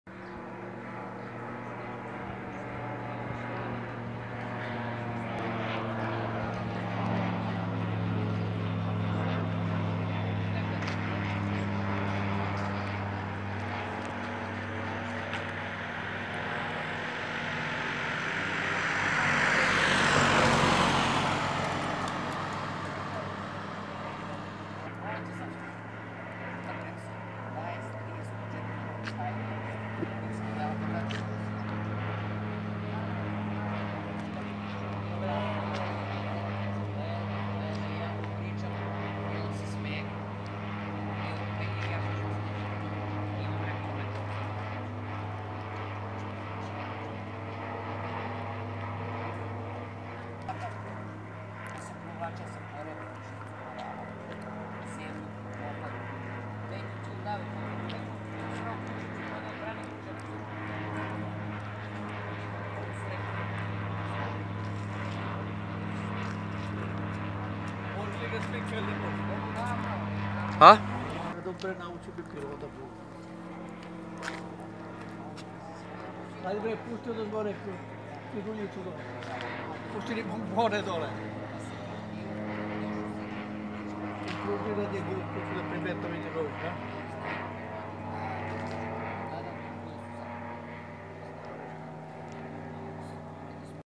Let aviona iznad Svrljiga
Moj dragi komšija Nemanja pilotira avionom iznad Svrljiga...Hvala mu za ovaj let.